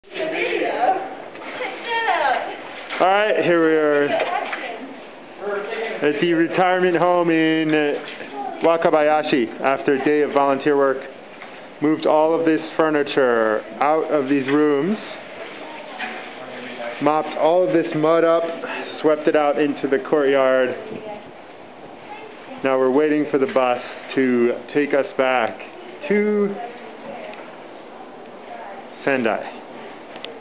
Wakabayashi Retirement Home Volunteering #1.AVI
On April 5th, three of us went out to Wakabayashi, Sendai to do some volunteer work cleaning up a retirement home.  Here's a video summarizing the day's work.